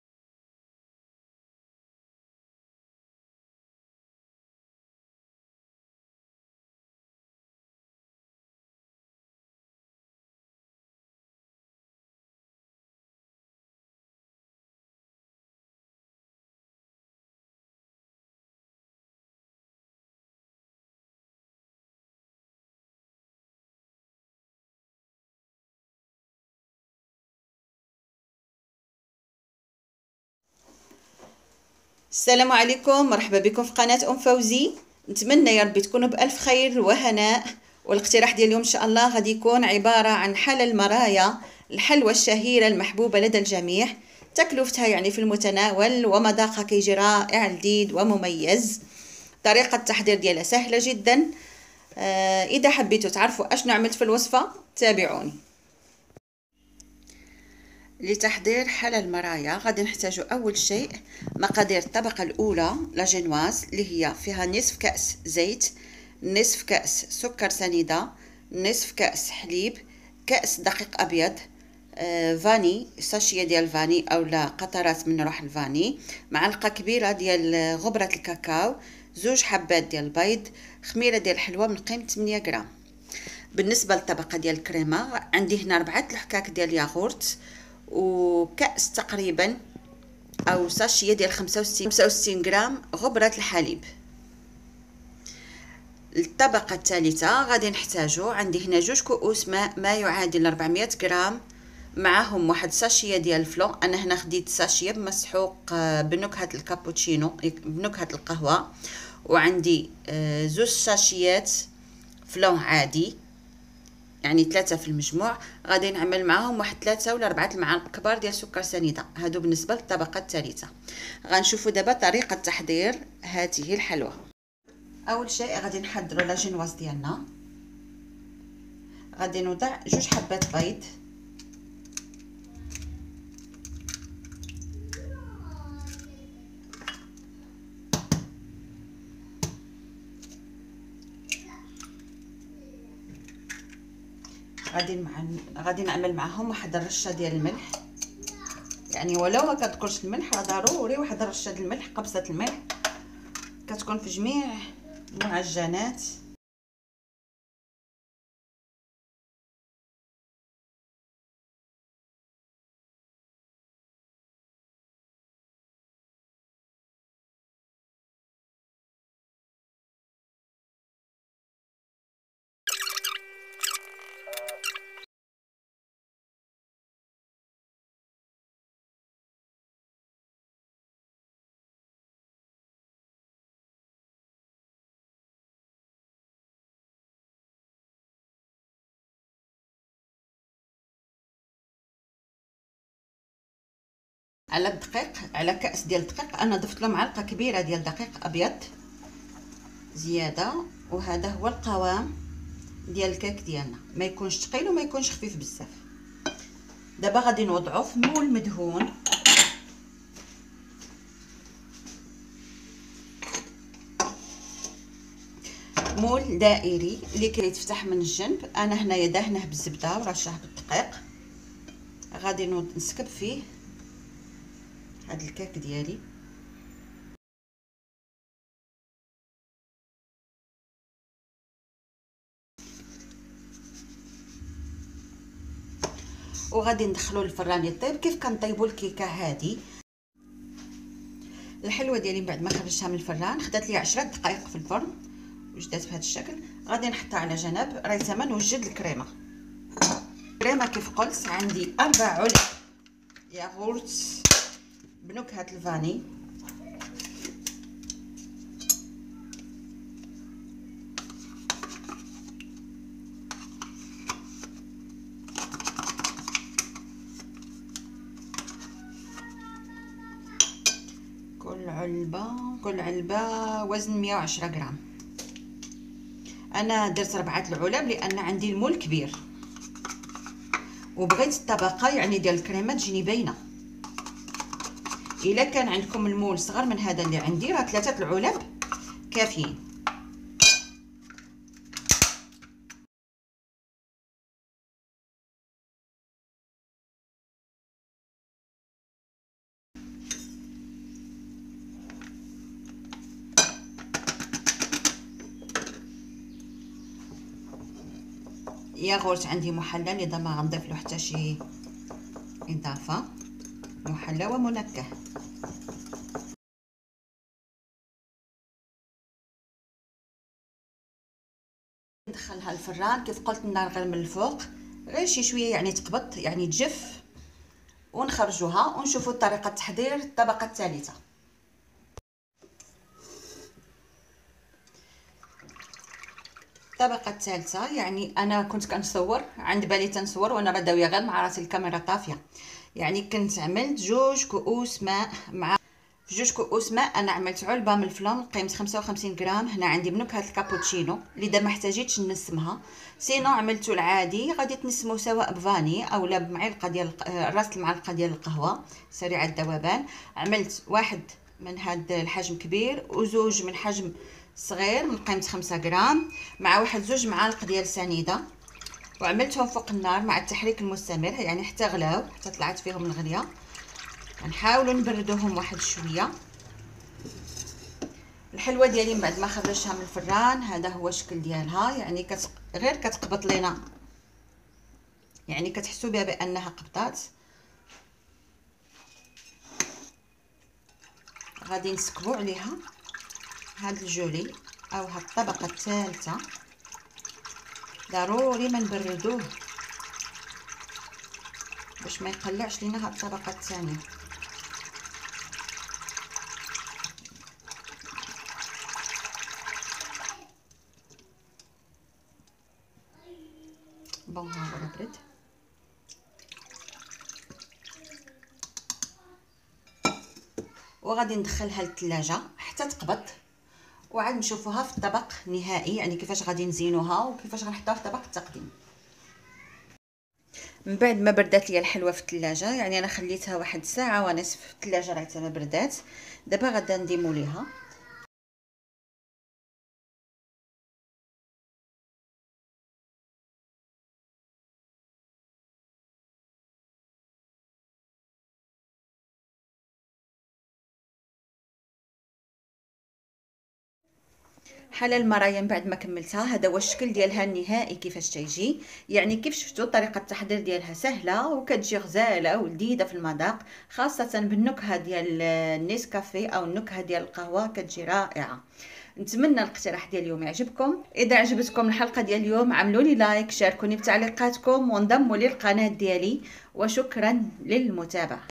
حلا المرايا
طريقة تحضير حلا المرايا الحلوة المحبوبة لدى الجميع
تكلفتها في المتناول وطريقة تحضيرها سهلة جدا ومذاقها رائع ومميز
نحتاج للطبقة الأولى
2حبات بيض مع نصف كأس سكر سنيدة ونصف كأس زيت ونصف كأس حليب رشة ملح ونكهة الفاني وممكن نضيفو قليل من القهوة
نخلط الكل بالتتالي ثم نضيف كأس دقيق مع معلقة كبيرة دقيق وخميرة حلويات ومعلقة كبيرة كاكاو نحصل على خليط الكيك نسكبه في مول يفتح من الجانب
وندخله الفرن مدة 10 إلى 15 دقيقة

الطبقة الثانية
4 علب ياغورت بنكهة الفاني و65 غ غبرة الحليب نخلط الكل ونسكبه على الكيك طبعا بعد ما يبرد وندخله الفرن النار من الفوق فقط لتنضج طبقة الكريمة مدة 8 إلى 10 دقائق  ثم نتركه يبرد ونحضر الطبقة الثالثة
الطبقة الثالثة
2 كؤوس ماء مايعادل 400 غ مع فلان حجم كبير بنكهة القهوة وفلان صغار من قيمة 5غ مع 2معالق إلى 3 معالق سكر
نخلط جيدا ونوضعهم على النار مع التحريك حتى الغليان ثم نتركه يبرد ونسكبه فوق طبقة الكريمة
تدخل الثلاجة حتى تبرد وتزين حسب الرغبة